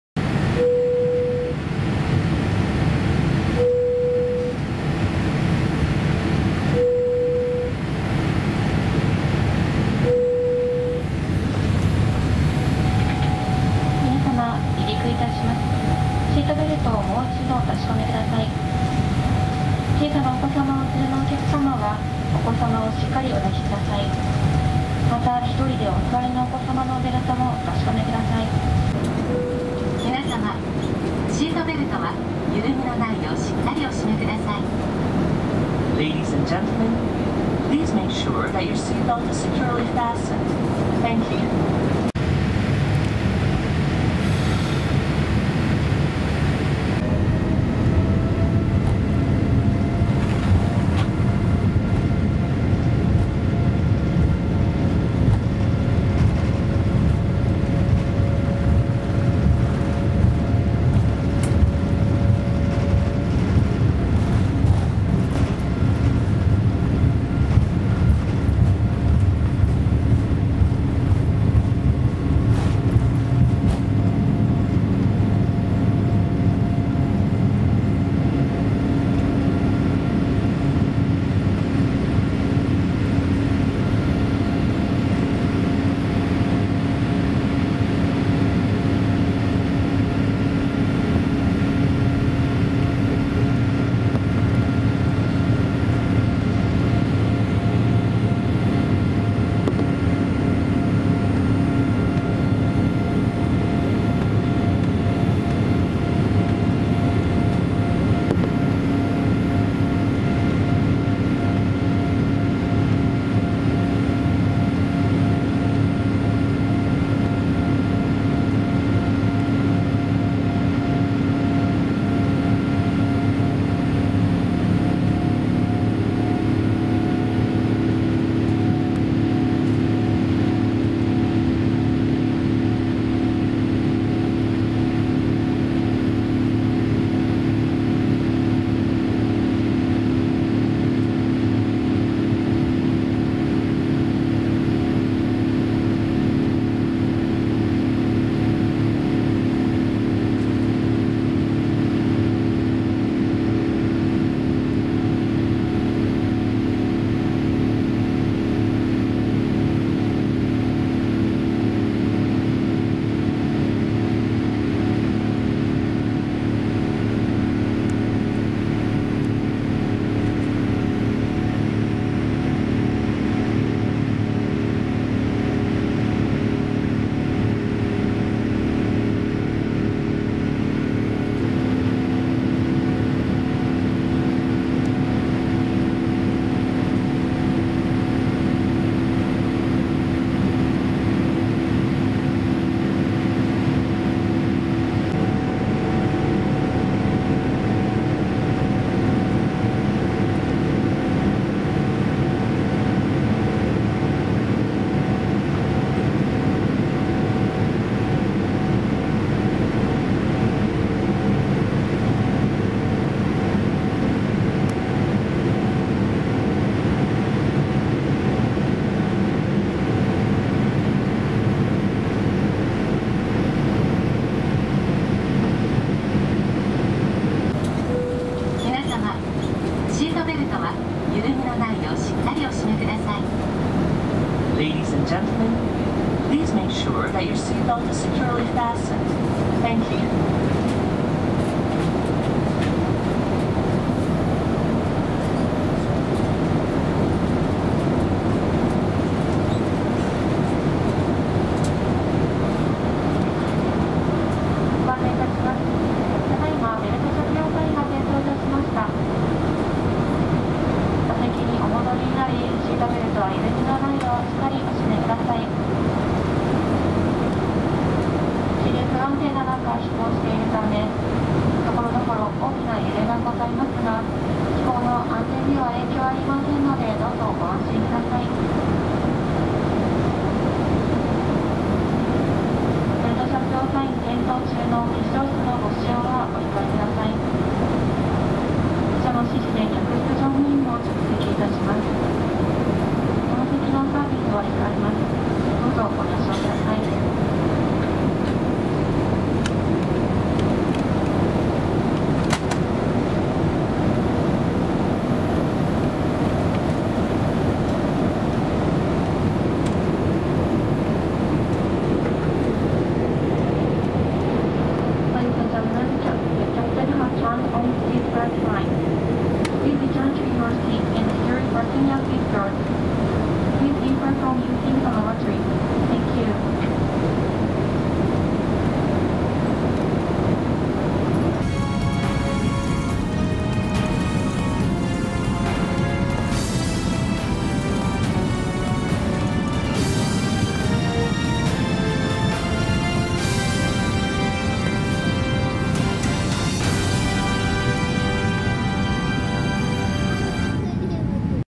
【気流不安定】羽田空港D滑走路離陸／ANAプレミアムクラス羽田空港→那覇空港
2021年9月26日撮影、ANA993便羽田空港→那覇空港Boeing787-8国際線機材(JA878A)プレミアムクラスより撮影した羽田空港D滑走路離陸～気流不安定映像です。

【撮影機材】
Panasonic 4K VZX990M 64GB HC-VZX990M-W
4K ビデオカメラ VZX992M

Everyday Update!